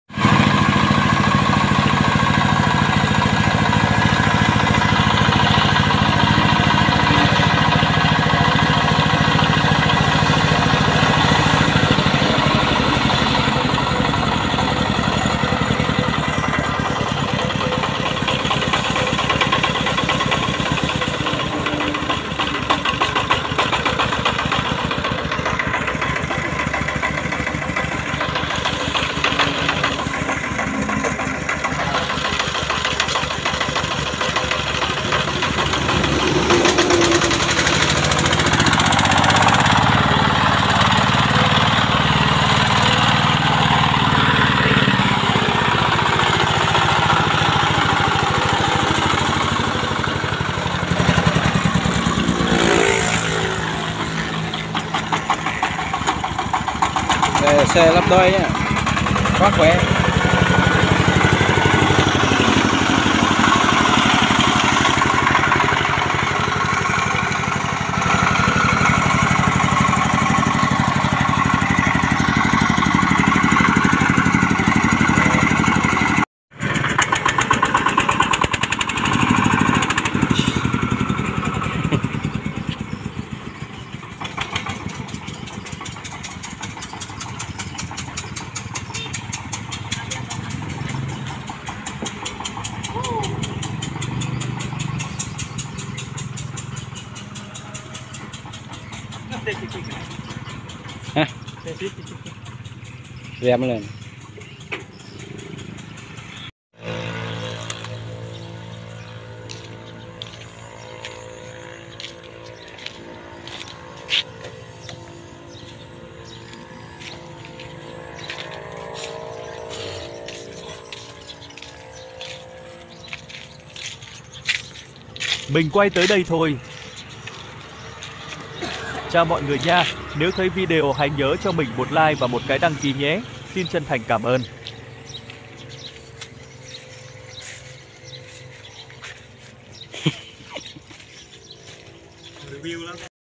Xe công nông 2 cầu đẩy lốp đôi siêu khỏe
Nay Nhương 81gl, do thời tiết mưa gió nên chúng tôi phải cắt bỏ dọn dẹp lại mới cây mà có khả năng ảnh hưởng đến trường học hay đường xá, để phòng chống khi bảo và mưa to, tránh ảnh hưởng đến trường và học sinh đến trường. Nếu video có ý nghĩa thì hãy cho mình 1like 1 cái đky nhé.